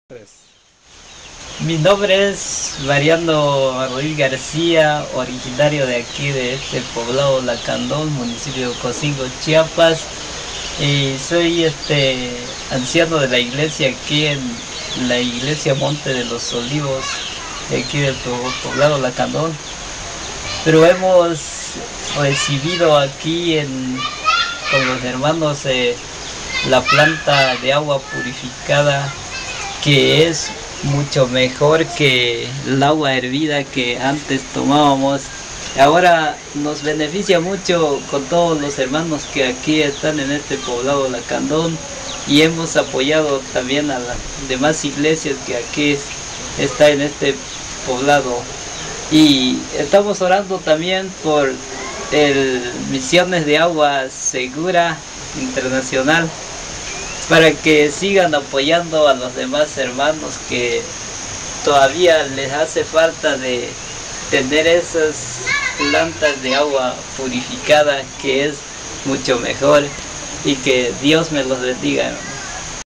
Lacandon (Ocosingo, Chiapas - Mx) - Testimonial by Mariano Ruíz García (Beneficiary).mp4
Testimonial by beneficiary during 1st quarterly visit